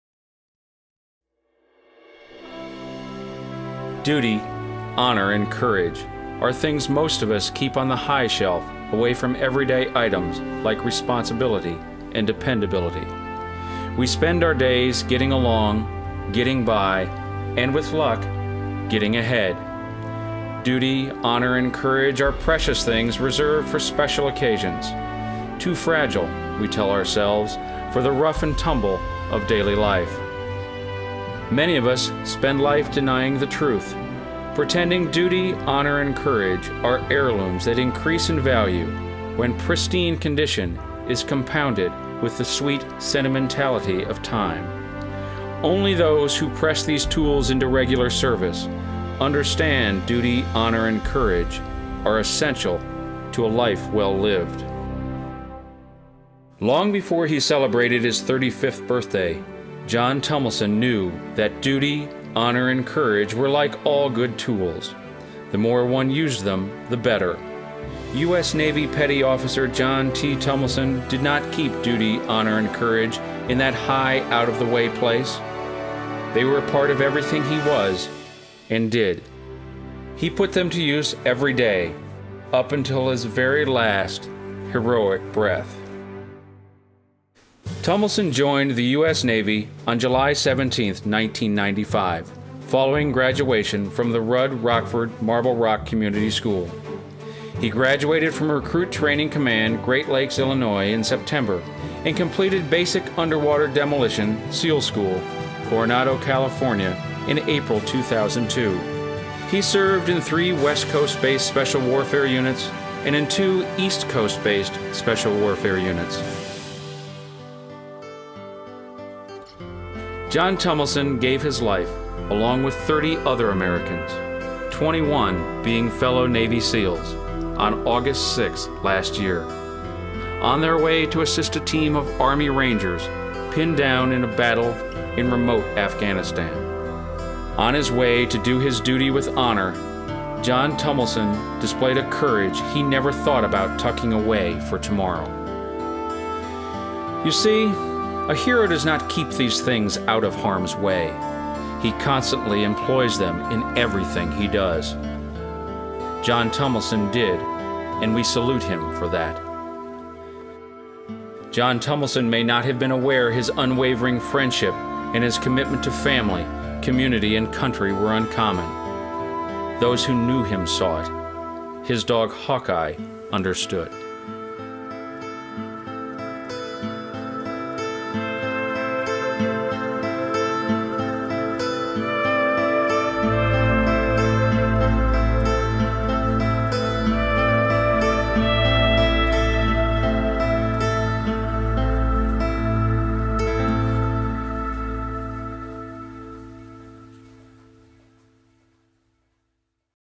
Red Cross Tribute to Jon Tumilson, April 12, 2012
This video was produced to honor US Navy Petty Officer Jon T. Tumilson at the Red Cross of Central Iowa's 2012 Heroes of the Heartland.

In recognizing Tumilson, the Red Cross paid tribute to all of the men and women serving our country.

"What they did, what they do and what so many unsung heroes like them do is nothing short of breathtaking. They put the very best of the human spirit to work here on earth. Yet, even the remarkable people honored this morning stand in awe at those giants among us who serve in uniform as they go silently about the work of preserving freedom and human dignity at home and in the darkest corners of the world."